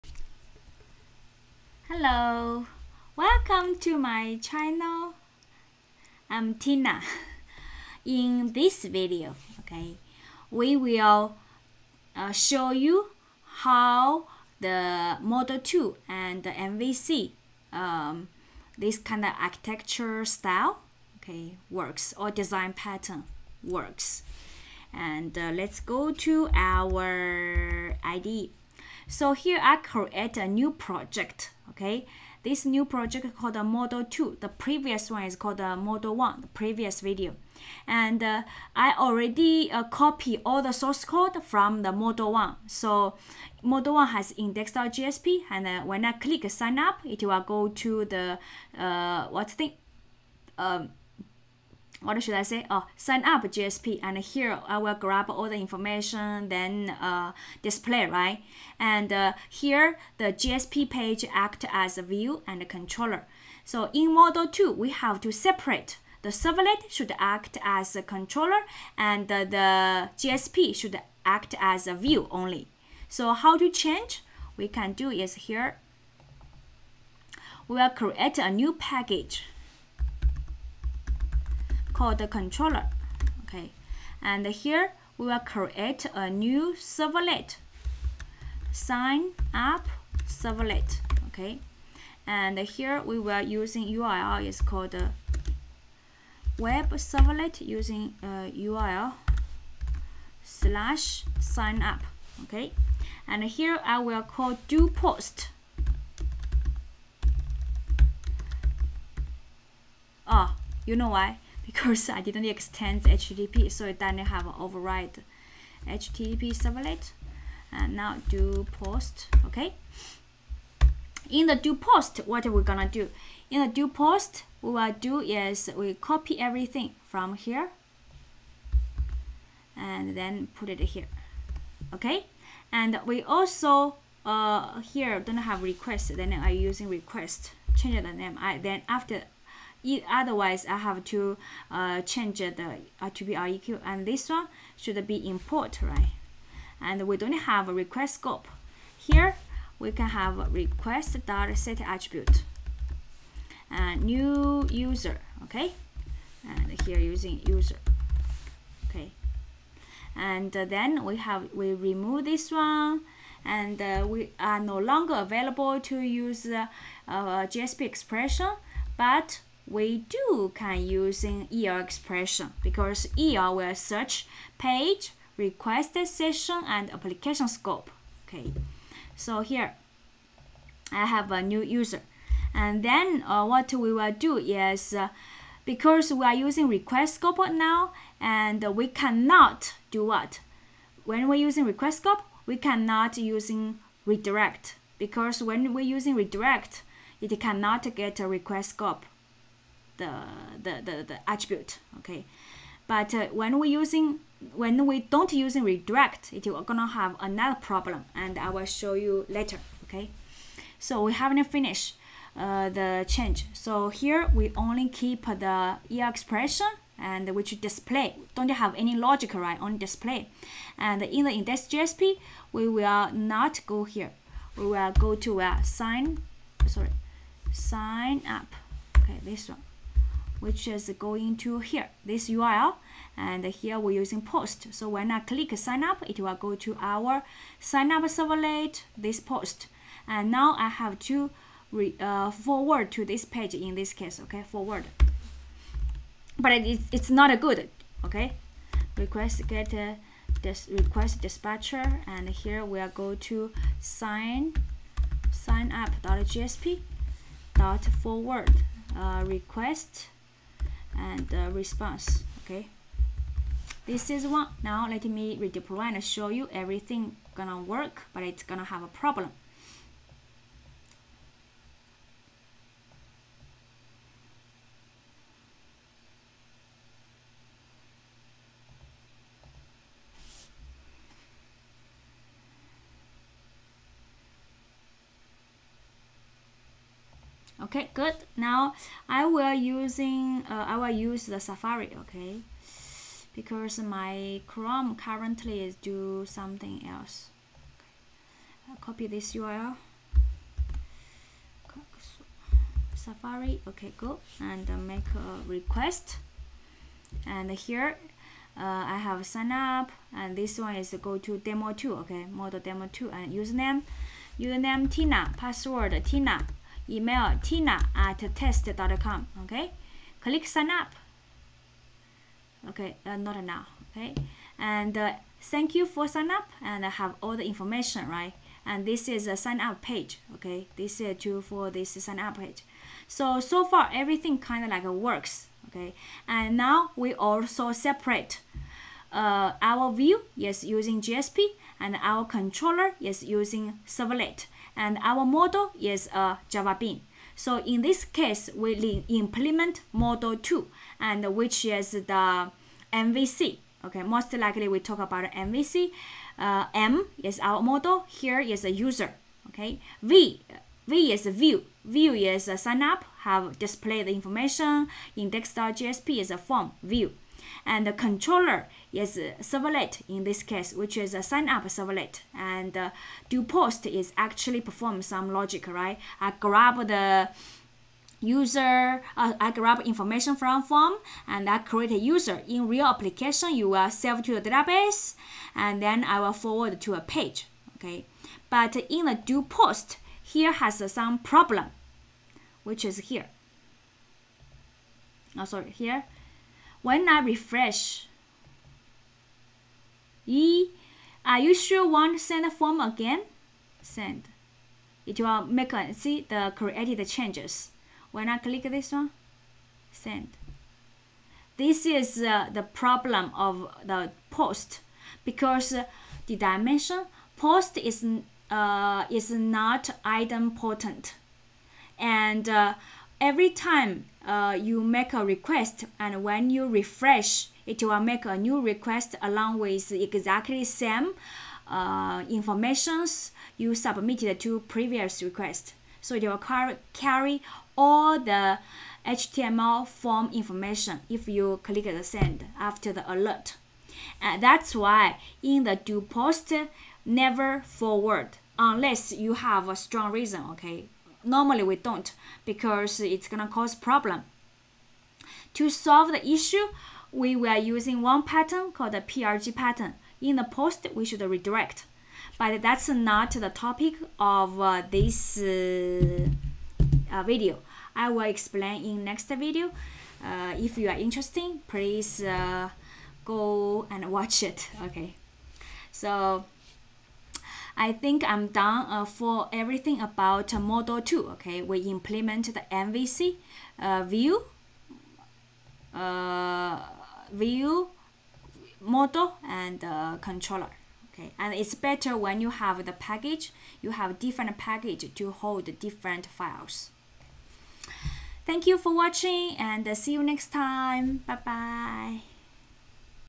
JSP Model 2 or MVC Demo
In this video, shows you how to convert JSP Model1 project using MVC.